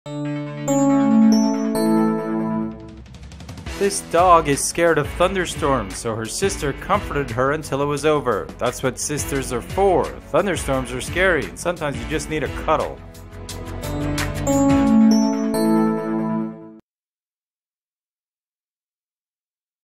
This Dog Is Scared of Thunderstorms, So Her Sister Comforted Her Until It Was Over
That's what sisters are for.